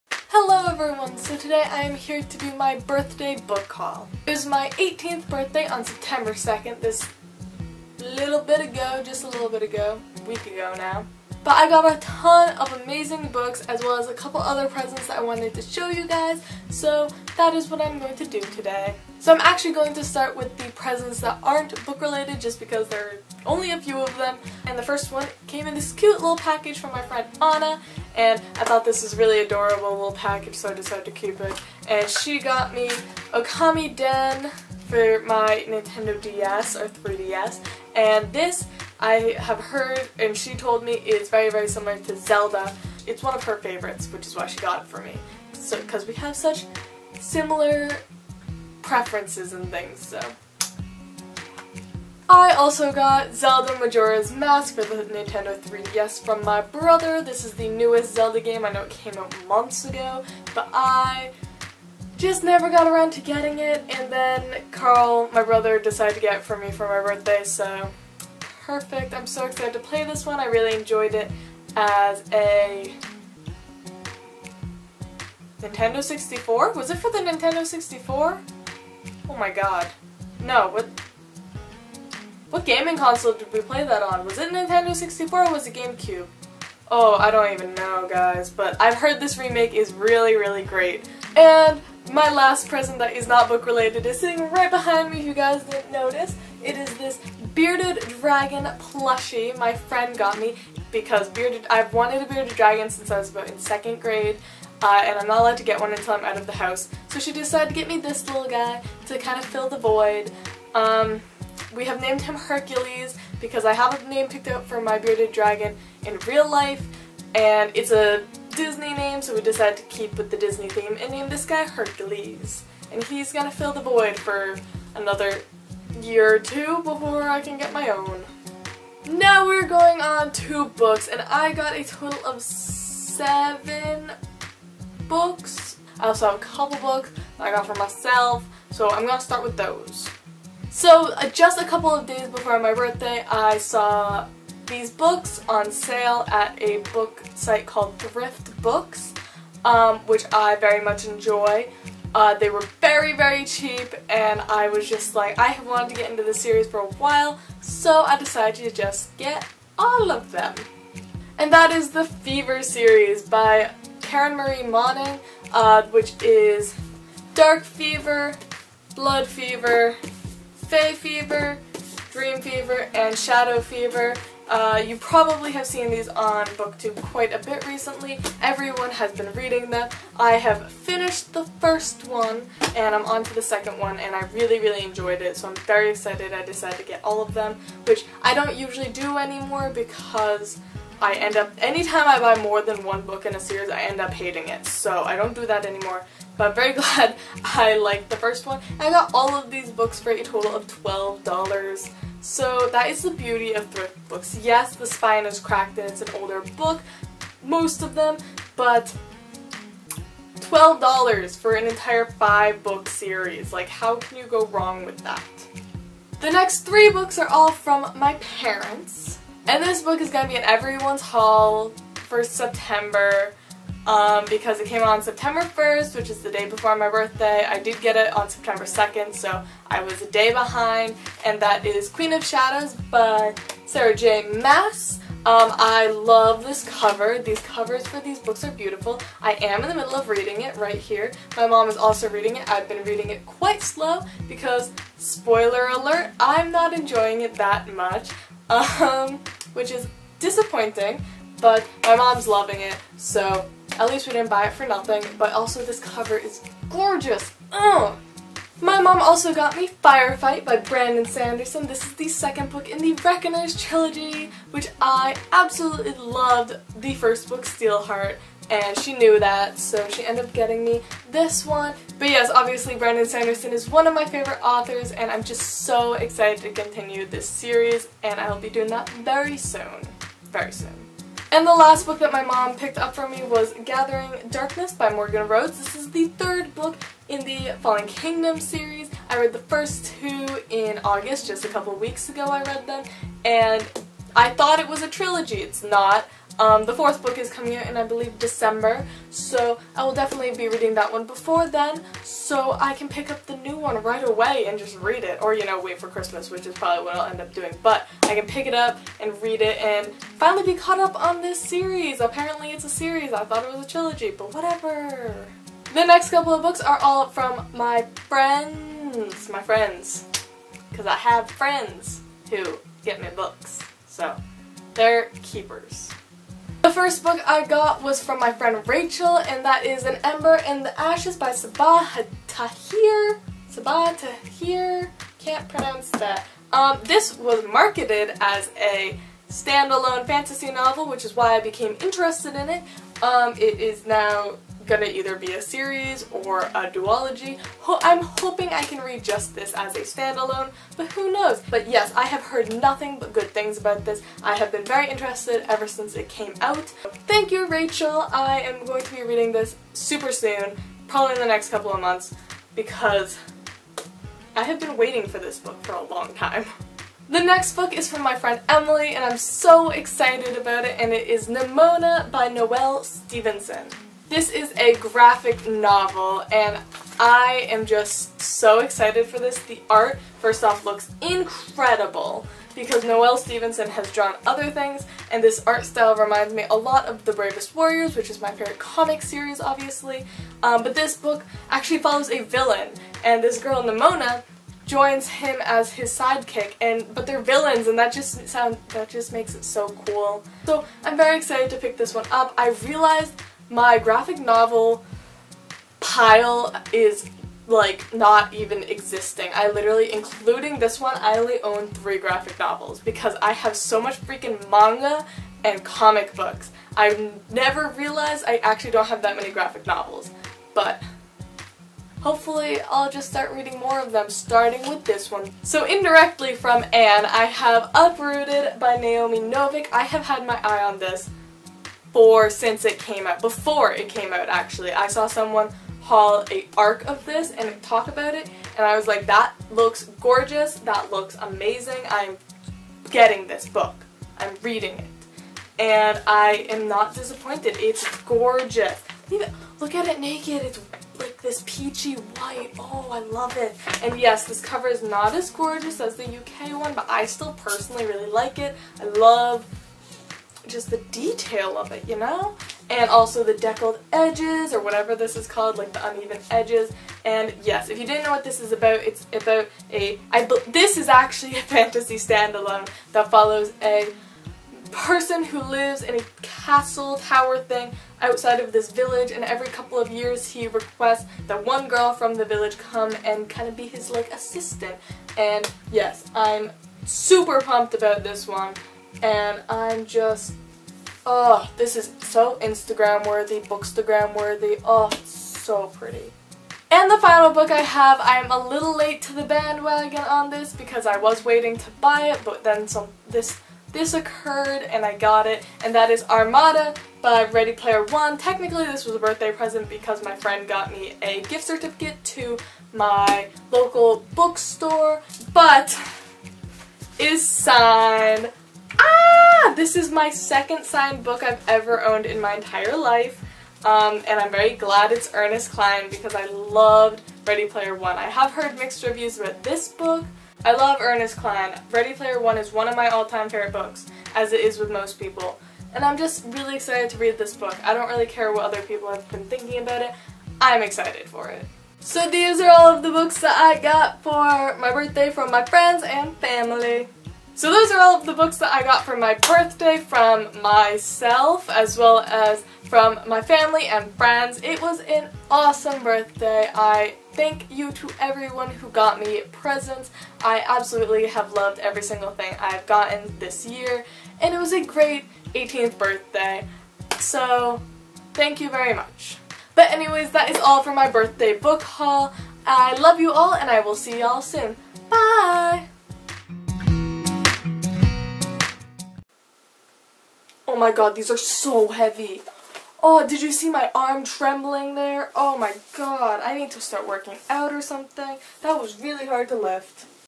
Birthday Bookhaul 2015!
My birthday was September 2 so OBVIOUSLY I GOT BOOKS